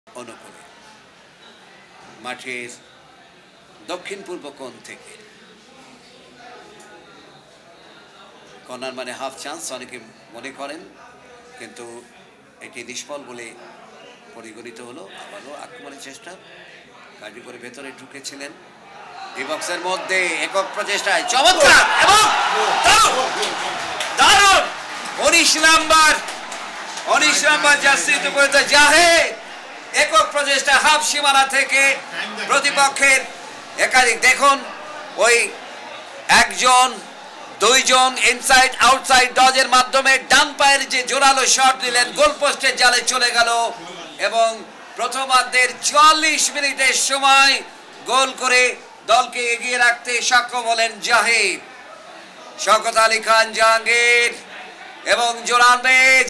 Legendary Zahid Hossain's Amazing goal.
Legendary Zahid Hossain scored this Amazing goal against Maldives in 2010 South Asian Games Football.